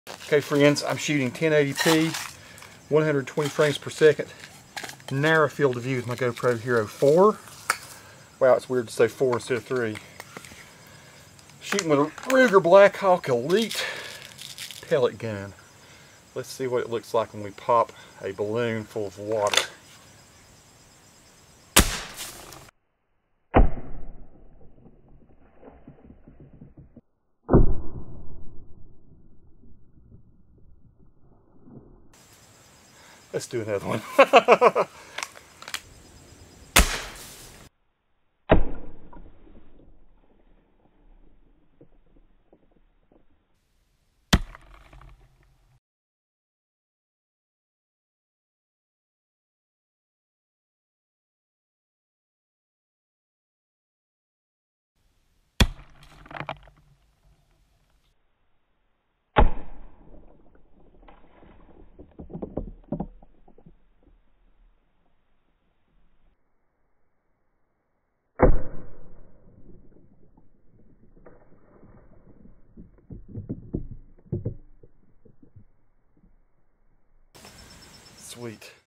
GoPro Hero4 - Shooting Water Balloons with Air Rifle 1080p 120fps
Me shooting some water balloons at 1080p, 120 frames per second with the GoPro Hero4 Black Edition. The air rifle is a Ruger Blackhawk Elite.